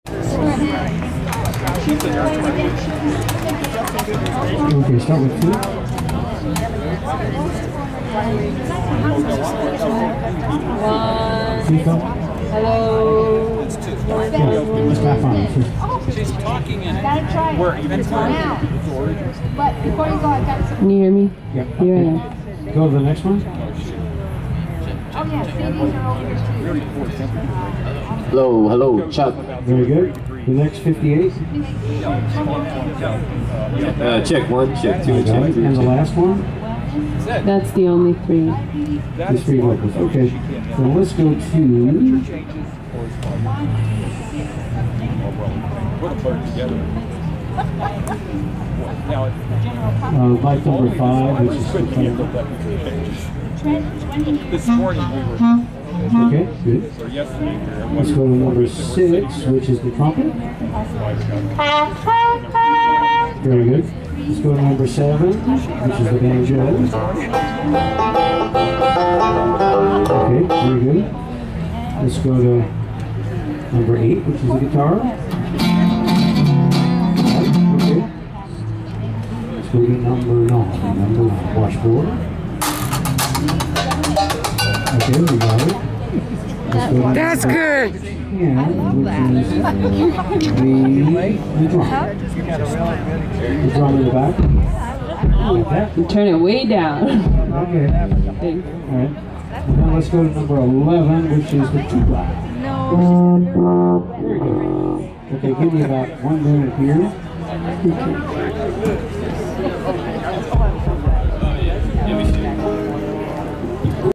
Tuba Skinny - California Worldfest
Tuba Skinny at The California Worldfest in Grass Valley, CA, July 18, 2015
I had the pleasure of meeting the group while they are on their west coast tour.  This is a fan's look at their pre-concert sound preparation; no music in the video.  No trombone player that day.